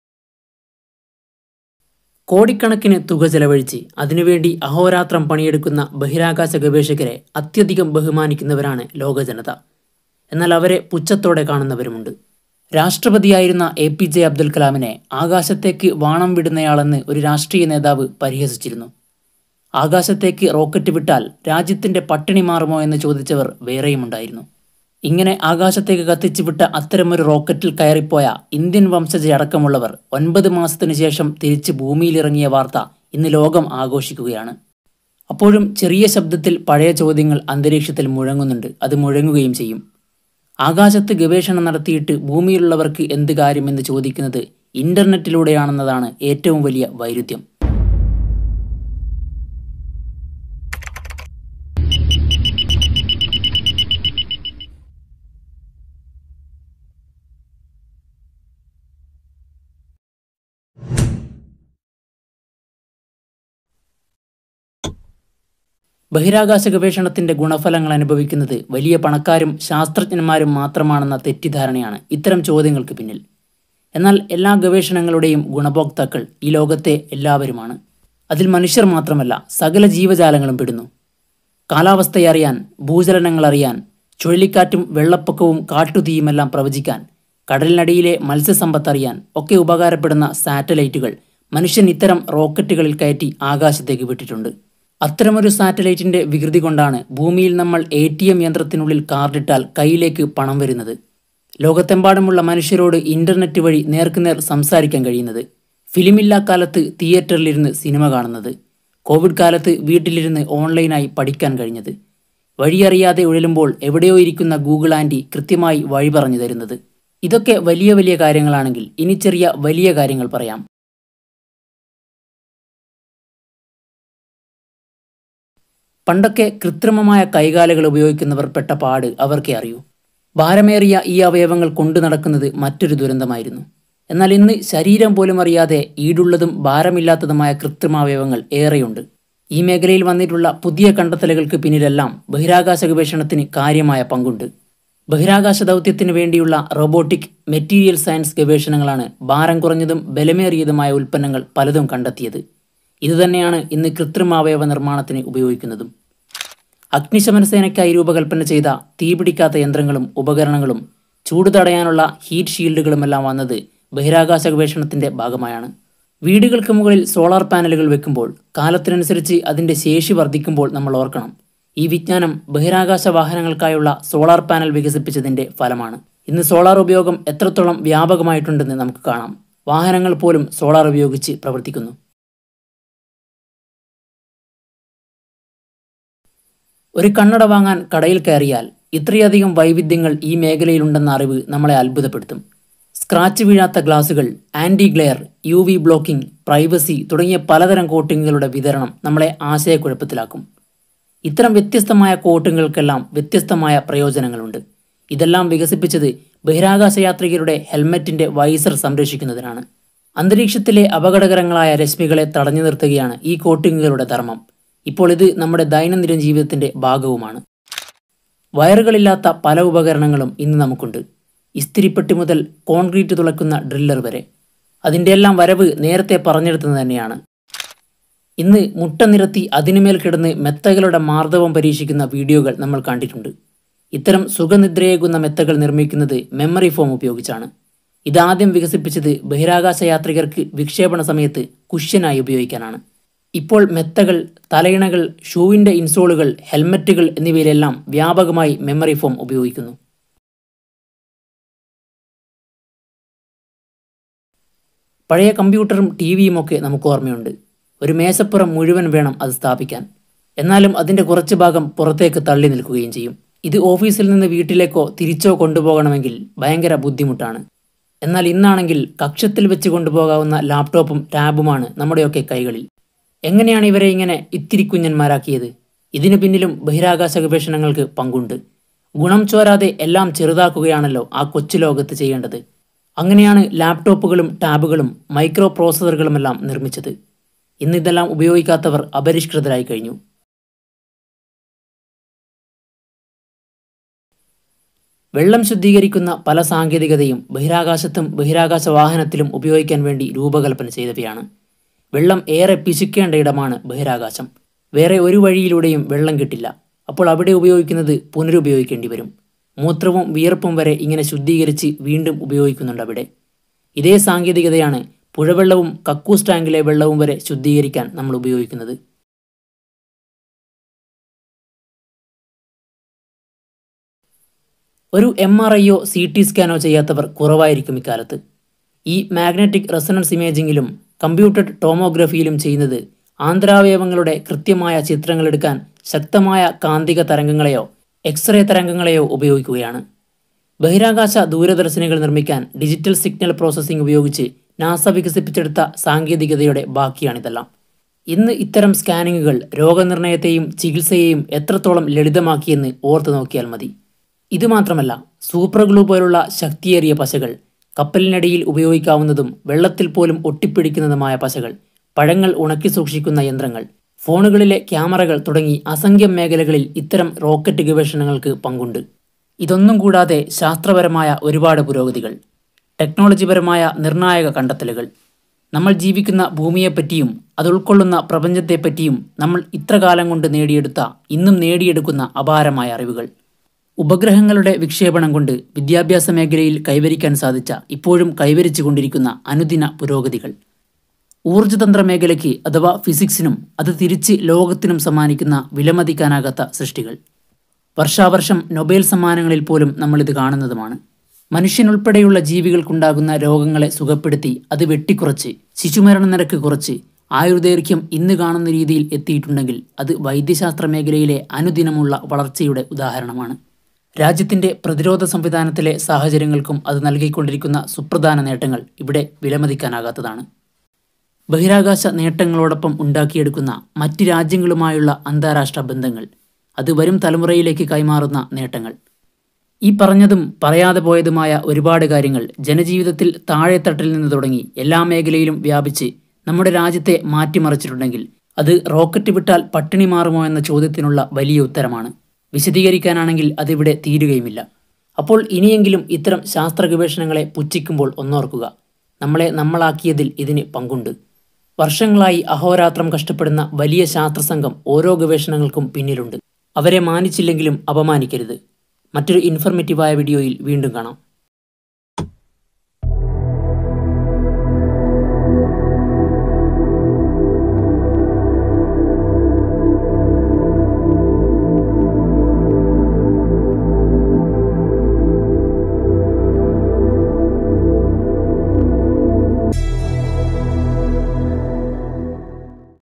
ഈ സംശയം നിങ്ങൾക്കുണ്ടോ..?| Space Research Advantages to the society
ആകാശത്തേക്ക് റോക്കറ്റ് വിട്ടാൽ നമുക്കുള്ള ഗുണങ്ങൾ | Space Research Advantages to the society

ashwinraj_m_r